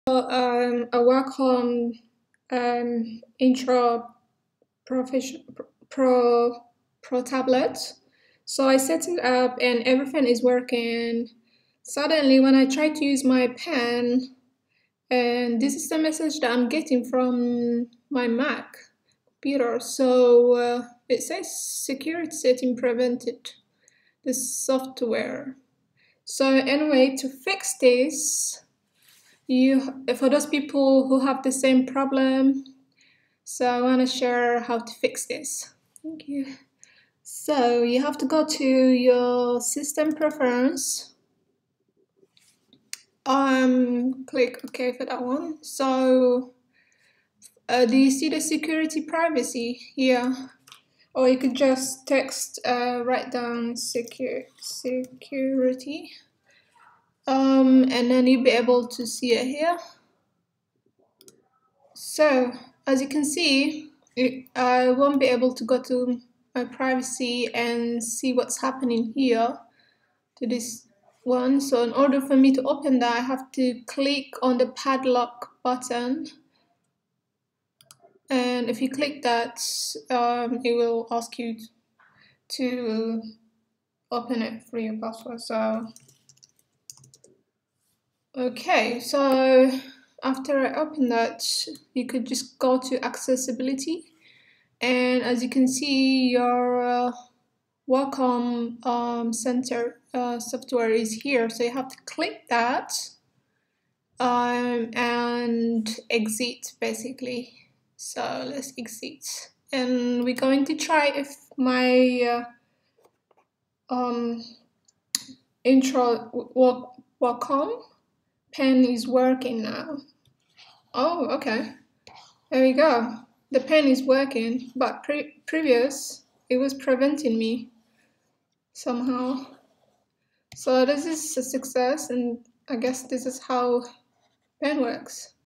How to fix your security Mac if your Wacom pen is not working on the tab
Hi there,  this video is for those people who set their Wacom up and unable to use the Wacom  pen on the tablet. As you can see a warning sign from my   Mac, I was unable to use my Wacom pen!